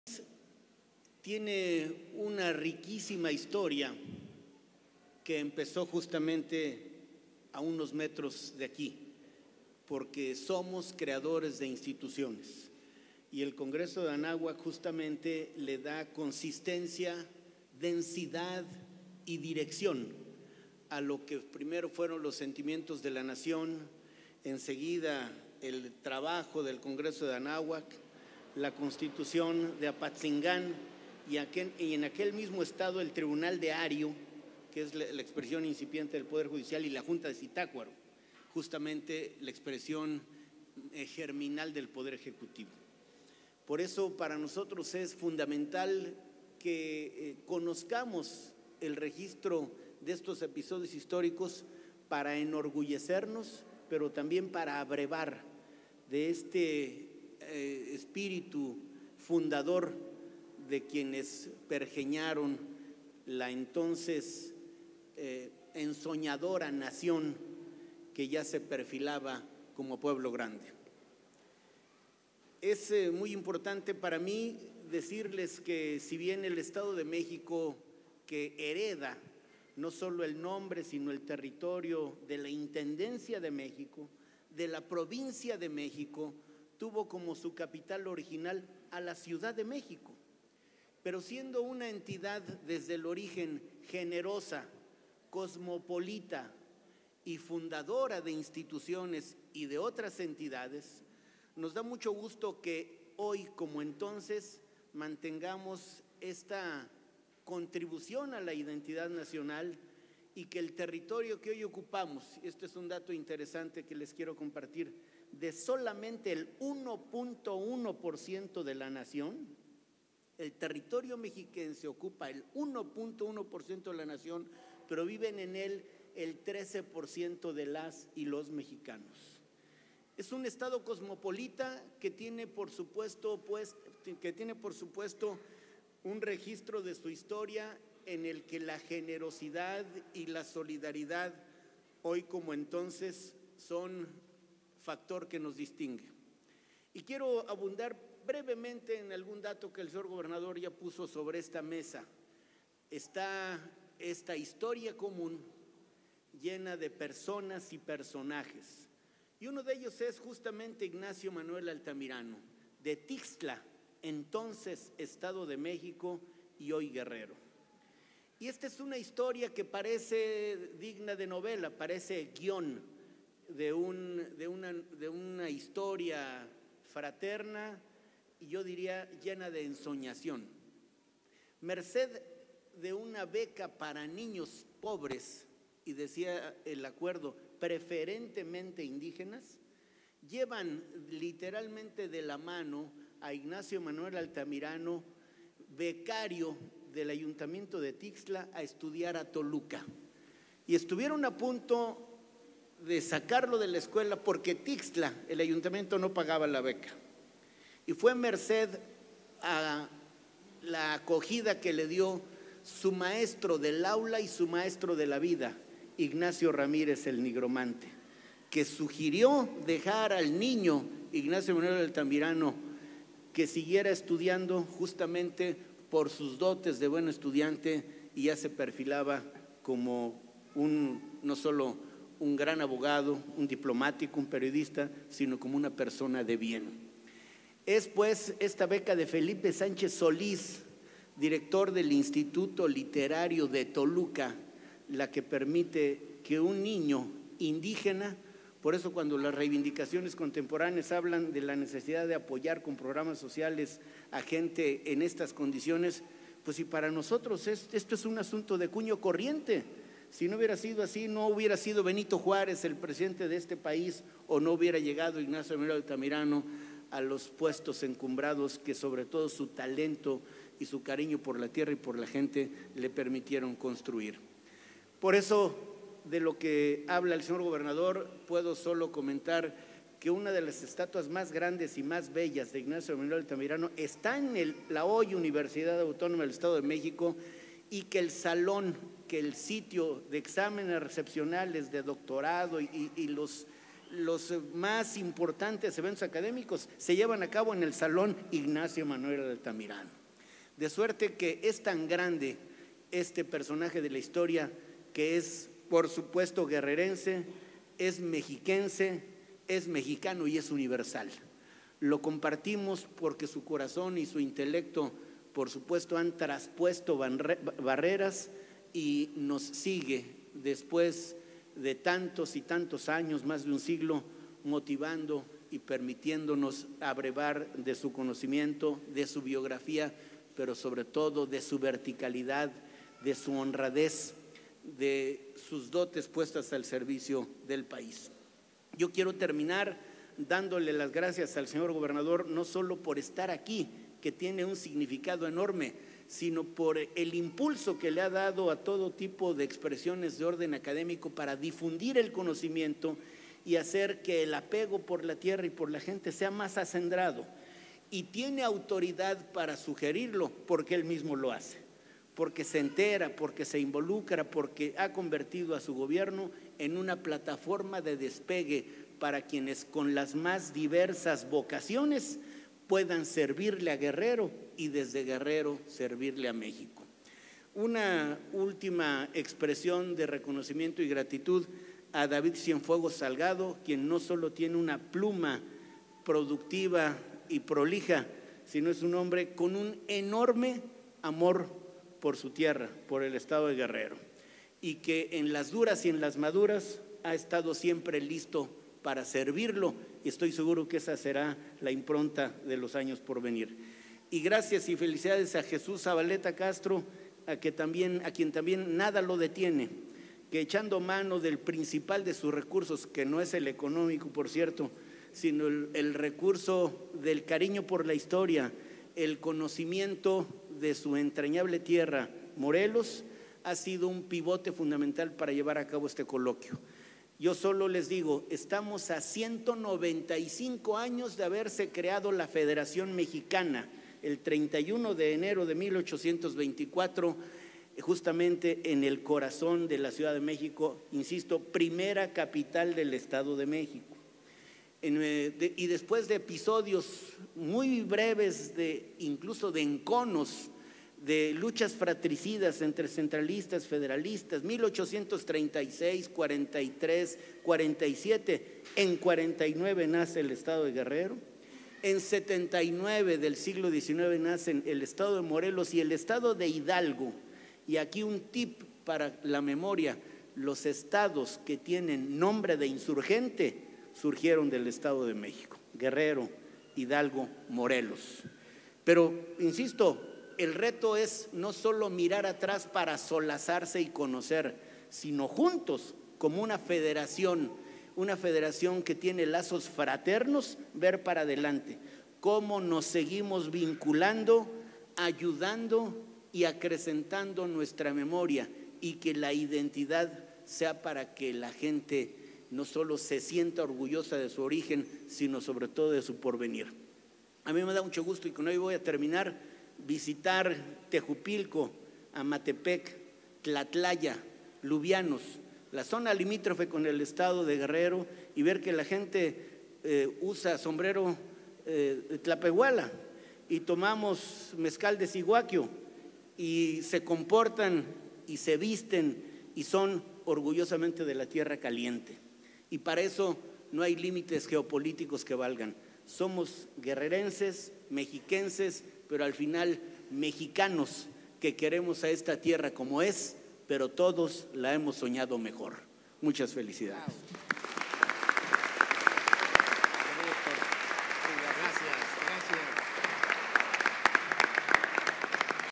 GradoCeroPress Mensaje César Camacho Quiroz, presidente del Colegio Mexiquense AC
INAUGURA GOBERNADOR HÉCTOR ASTUDILLO EL COLOQUIO: "MEMORIA, IDENTIDAD Y TERRITORIO"

* Este intercambio académico entre el Estado de México, Guerrero y Morelos busca lograr la vinculación histórica de estas tres entidades

* El Ejecutivo guerrerense expresó que la patria se formó en estas tierras, si bien Hidalgo inició una lucha, ésta se consolida en Chilpancingo"

CHILPANCINGO.- Con la presencia de investigadores y catedráticos del Estado de México, Guerrero y Morelos, el gobernador Héctor Astudillo Flores inauguró el coloquio "Memoria, identidad y territorio", con el que se logra una vinculación y un hermanamiento histórico entre estas tres entidades.

En un esfuerzo colectivo, este intercambio académico se realizará en tres sedes, la primera fue esta tarde en Chilpancingo, posteriormente se realizará en Cuernavaca y Toluca.

Durante este coloquio se habló sobre los lazos que unen a estos estados y los héroes de la historia de México que compartieron estos territorios como Morelos, Juan Álvarez, Vicente Guerrero e Ignacio Manuel Altamirano.

Se invitó a reflexionar sobre el pasado, pero también el presente del sur que ha dado cuotas enormes a esta nación, pero con la que aún se tienen pendientes.

Al hacer la inauguración oficial y a manera de anécdota, el Ejecutivo guerrerense resaltó que durante mucho tiempo, los del Estado de México reclamaron los nacimientos de los héroes, porque la única que existía era la provincia de Tecpan, así que Chilpancingo y Tixtla pertenecían al estado de México.

"La patria se formó en estas tierras, si bien Hidalgo inició una lucha, esta se consolida en Chilpancingo", afirmó.

El presidente del Colegio Mexiquense AC. César Camacho Quiroz, manifestó que es fundamental conocer el registro histórico de quienes tuvieron este espíritu fundador de la nación.

Reconoció que es importante hablar de la historia que unió a personajes de estas entidades como los Sentimientos de la Nación y el Primer Congreso de Anáhuac.

Pero también el Estado de México y Guerrero comparten a Ignacio Manuel Altamirano, guerrerense que también hizo vida académica y política en el Estado de México.

"Lo compartimos porque su corazón e intelecto traspasaron barreras y motiva a abrevar de su biografía, de su honradez y sus dotes que fueron puestos al servicio del país".

Camacho Quiroz, indicó que con este tipo de vinculaciones académicas se refuerzan los lazos fraternos y se fortalece la memoria, la identidad, para que la gente se sienta orgullosa de su origen y de su porvenir.

El catedrático de la Universidad Autónoma de Guerrero, David Cienfuegos Salgado, manifestó que estos eventos también se hacen en el marco de los próximos aniversarios de la creación de Morelos y Guerrero como entidades federativas.

En este evento también estuvieron el presidente de la Mesa Directiva del Congreso del Estado, Alberto Catalán Bastida, así como los investigadores Jaime García Mendoza, José Martínez Pichardo y Ricardo Infante Padilla y el presidente de la Sociedad para el Patrimonio Cultural A.C. Jesús Zavaleta Castro.